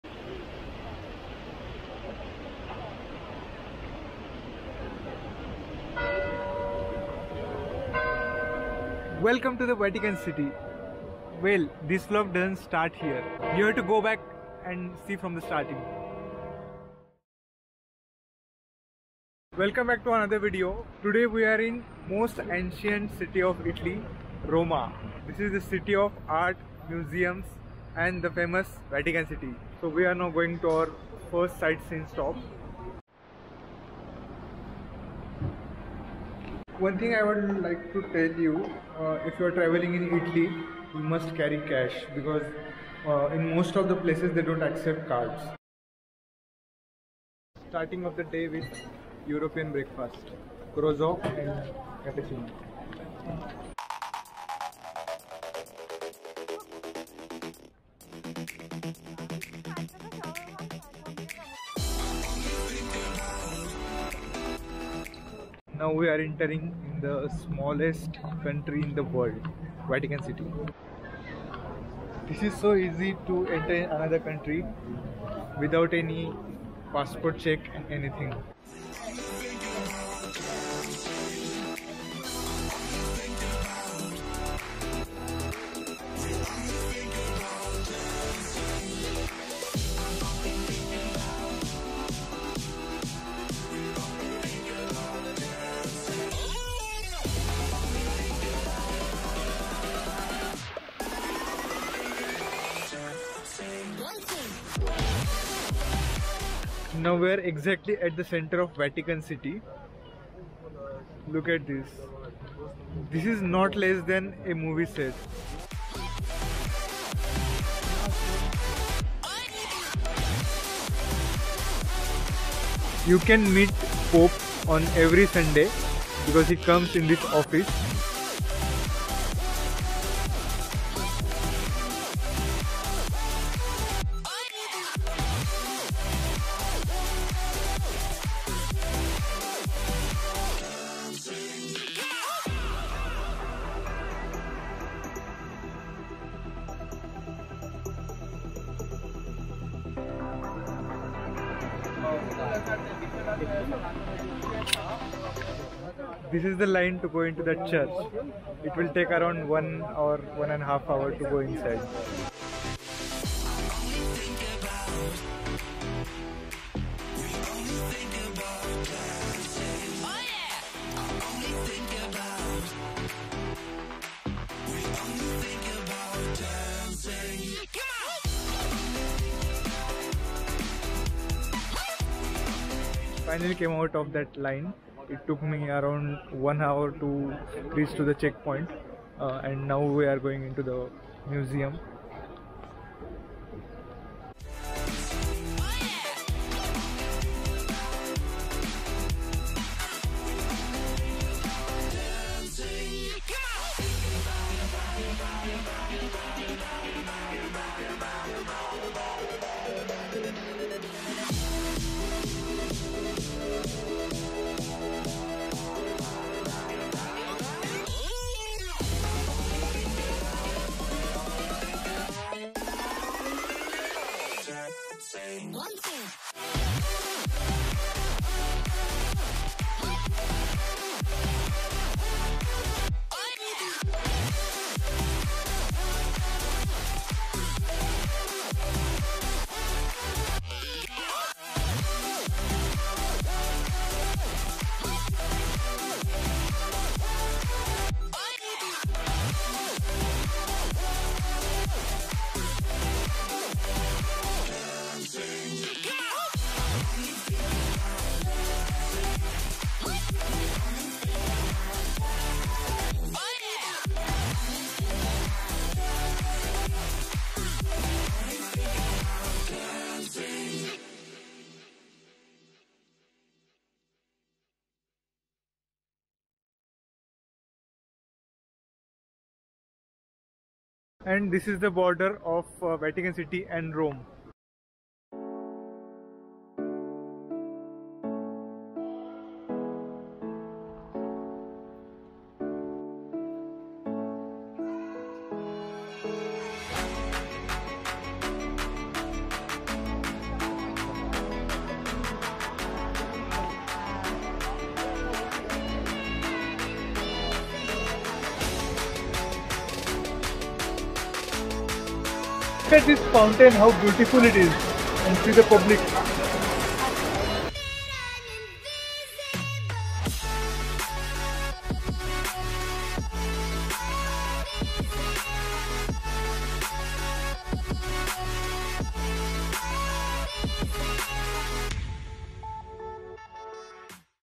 Facts about Worlds Smallest Country: Vatican City | Rome Italy | Vatican City Explained FREE ENTRY
While on the Italy trip, my third started with visiting the Vatican city followed by Trevi Fountain in Rome City.
Vatican City is the smallest country in the world.
Vatican City, a city-state surrounded by Rome, Italy, is the headquarters of the Roman Catholic Church. It's home to the Pope and a trove of iconic art and architecture. Its Vatican Museums house ancient Roman sculptures such as the famed “Laocoön and His Sons” as well as Renaissance frescoes in the Raphael Rooms and the Sistine Chapel, famous for Michelangelo’s ceiling.
The Trevi Fountain is a fountain in the Trevi district in Rome, Italy, designed by Italian architect Nicola Salvi and completed by Giuseppe Pannini and several others. Standing 26.3 meters high and 49.15 meters wide, it is the largest Baroque fountain in the city and one of the most famous fountains in the world.